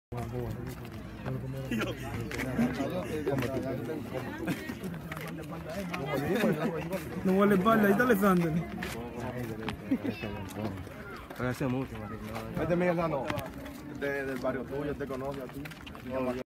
solpresa de miguel sano el pelotero de spm
visita a niños y adolecentes en spm